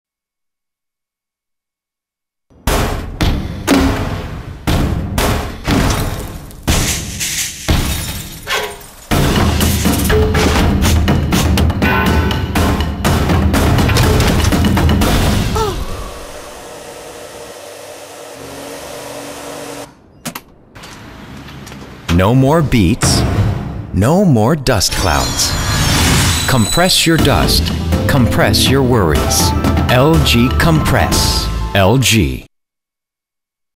LG Compressor Vacuum
What a wonderful vacuum !
It makes dust to cube !!